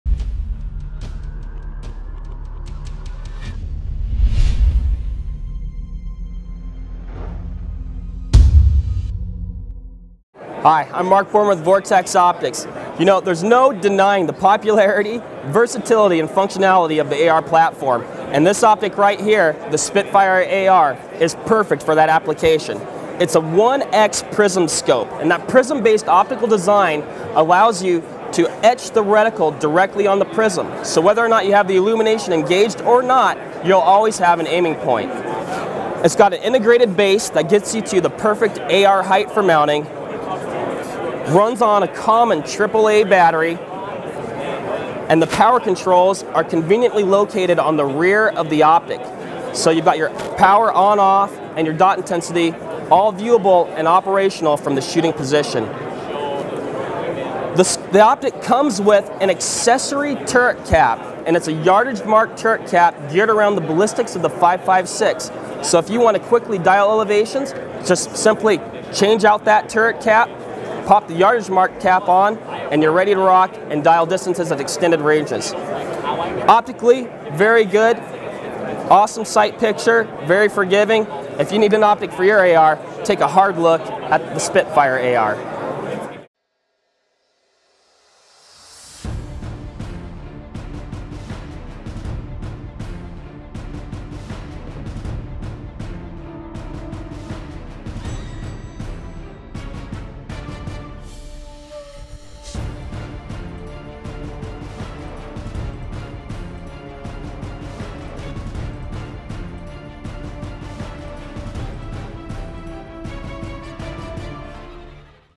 Vortex Spitfire AR Optic - SHOT Show 2016
Mark from Vortex shows us the new Spitfire AR from Vortex Optics.